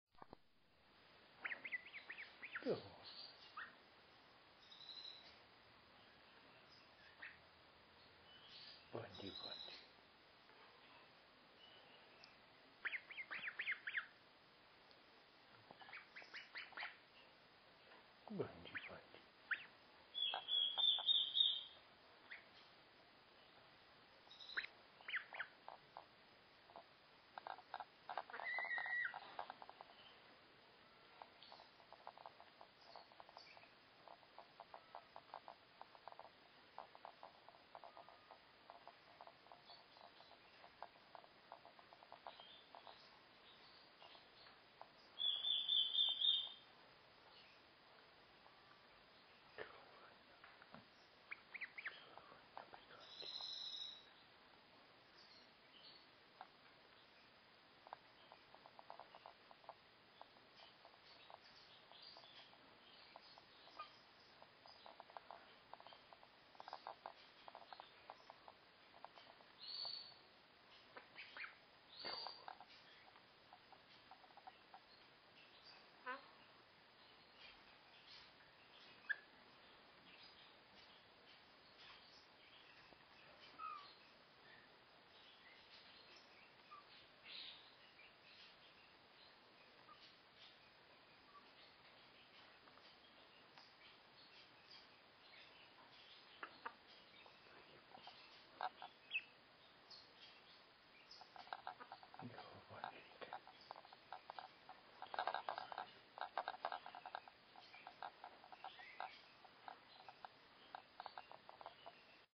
Hanyatt liba ölben, pocakszárítás közben
Most mit mondjak? :-)
Ő Lilike, bár a filmen rendszeresen lebandizom szegénykét, mert azt hittem, a tesója van az ölemben.
Kint voltunk a vizes fűben, vizes lett a pocakja, így meg kellett szárítani egy kis dajkálással egybekötve :-)
Lili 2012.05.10-én kelt, a felvételen 23 napos kis bütykös liba.